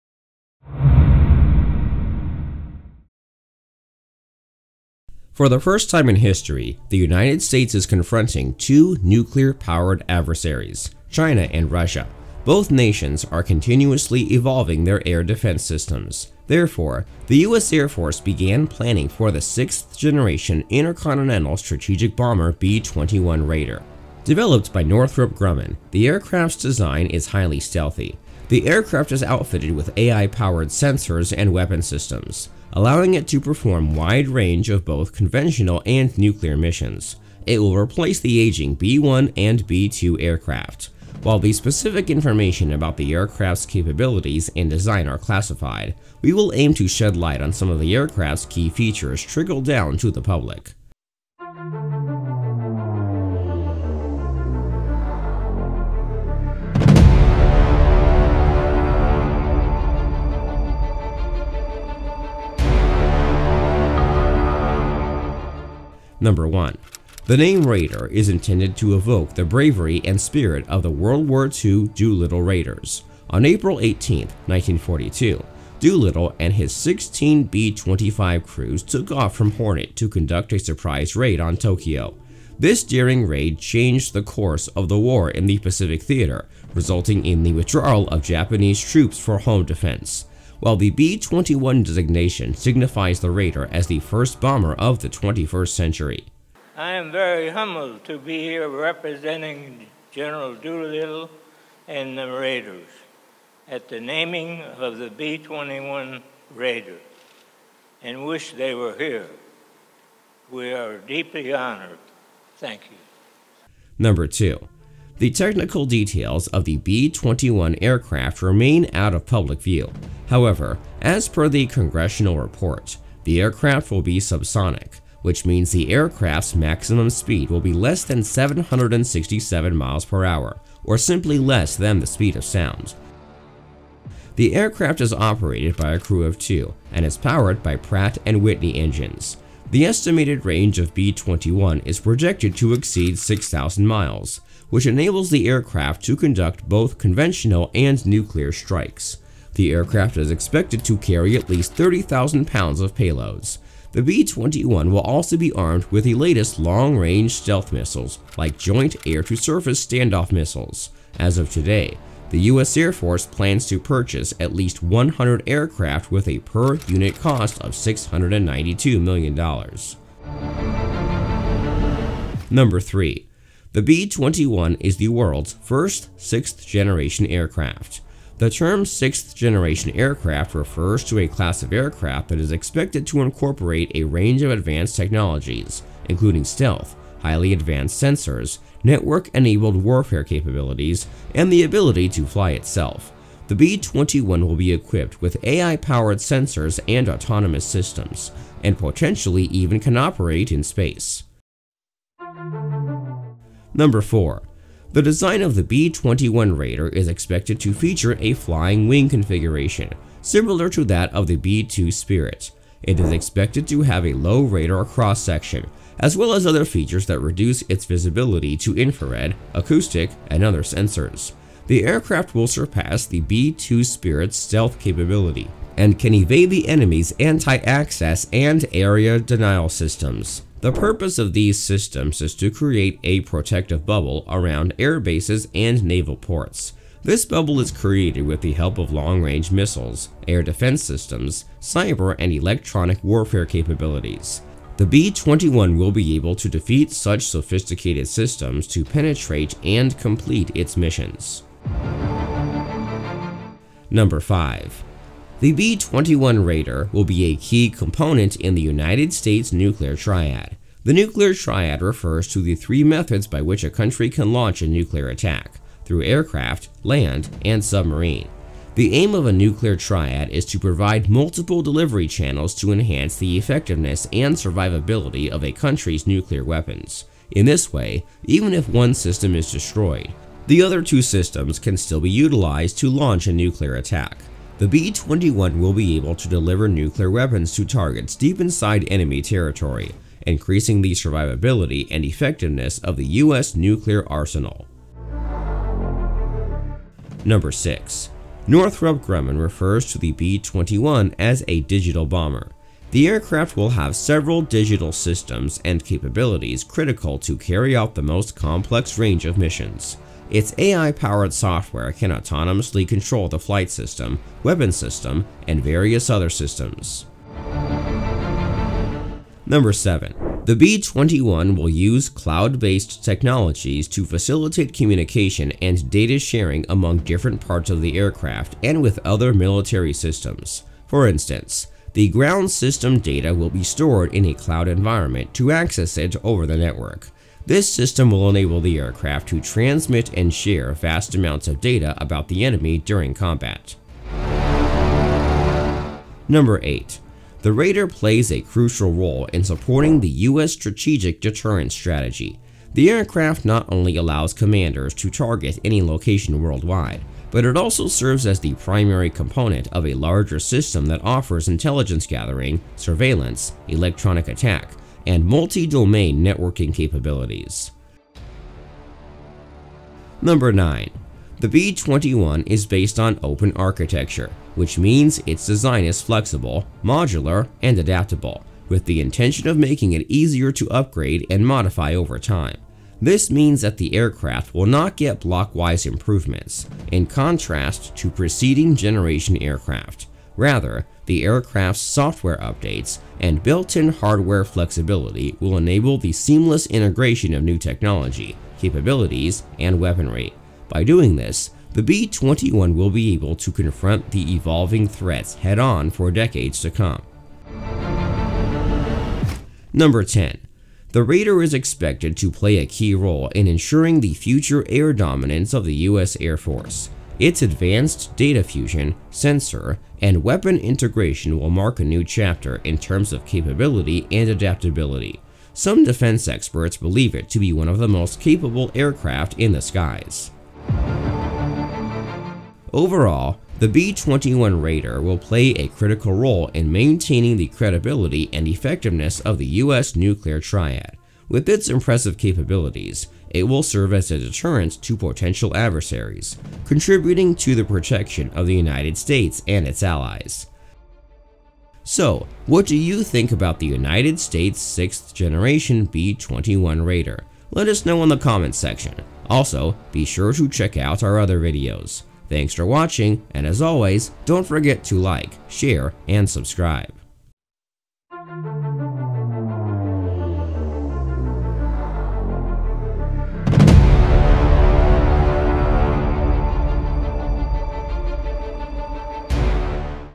B-21 Raider: Meet the World’s first 6th Gen Stealth Bomber
For the first time in history, the United States is confronting two nuclear-powered adversaries - China and Russia. Both nations are continuously evolving their air defense systems. Therefore, the US airforce began planning for the sixth-generation intercontinental strategic bomber B-21 Raider. Developed by Northrop Grumman, the aircraft’s design is highly stealthy. The aircraft is outfitted with AI-powered sensors and weapons systems, allowing it to perform wide range of both conventional and nuclear missions. It will replace the aging B-1 and B-2 aircraft. While the specific information about the aircraft’s capabilities and design are classified, we will aim to shed light on some of the aircraft’s key features trickled down to the public. The name "Raider" is intended to evoke the bravery and spirit of the WW II Doolittle Raiders. The aircraft will be subsonic, which means the aircraft’s maximum speed will be less than 767mph or simply less than the speed of sound. The aircraft is operated by a crew of 2 and is powered by Pratt & Whitney engines. The estimated range of B-21 is projected to exceed 6,000 miles, which enables the aircraft to conduct both conventional and nuclear strikes. The aircraft is expected to carry at least 30,000lb of payloads. The B-21 will also be armed with the latest long-range stealth missiles like Joint air-to-surface stand-off missiles. As of today, the US airforce plans to purchase at least 100 aircraft with a per unit cost of $692 million. The B-21 is the world’s first sixth-generation aircraft. The design of the B-21 Raider is expected to feature a flying wing configuration, similar to that of the B-2 Spirit. It is expected to have a low radar cross-section, as well as other features that reduce its visibility to infrared, acoustic, and other sensors. The B-21 Raider will be a key component in the United States' nuclear triad. The aircraft will have several digital systems and capabilities critical to carry out the most complex range of missions. Its AI-powered software can autonomously control the flight system, weapons system, and various other systems. The B-21 will use cloud-based technologies to facilitate communication and data sharing among different parts of the aircraft and with other military systems. The Raider plays a crucial role in supporting the U.S. strategic deterrence strategy. The B-21 is based on "open architecture", which means its design is flexible, modular, and adaptable, with the intention of making it easier to upgrade and modify over time. The Raider is expected to play a key role in ensuring the future air dominance of the US airforce.

Credits
1)US DOD
2)Northrop Grumman
3)Lockheed Martin
4)USAF

All content on Grid 88 is presented for only educational purposes. The appearance of US DOD, US Airforce, Northrop Grumman and Lockheed Martin does not imply or constitute the respective entities or this channel’s endorsement.